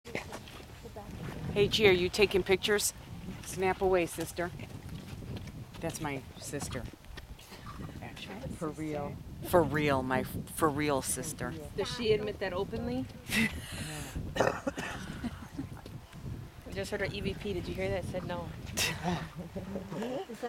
Paranormal Sisters
Having a little fun at the Cemetery Crawl with my fellow investigator...who is one of the funniest chicks in the paranormal field.